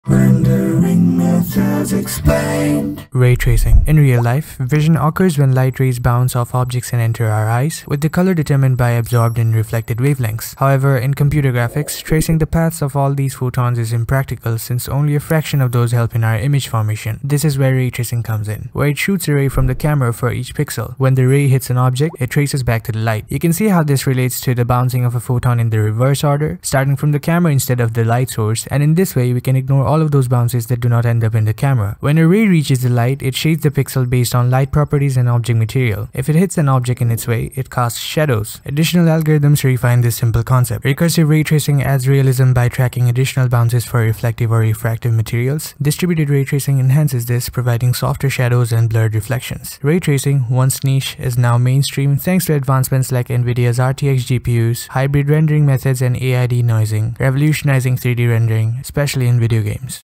Rendering Methods Explained: Raytracing
In this series, I'll give my best efforts to explain all kinds of rendering techniques in the least amount of time possible, while still giving a decent idea about how it works.

This is the third video in this series where I explain the fundamental concept behind ray tracing, along with additional refinements to the original concept that made it a huge step towards photorealistic renders.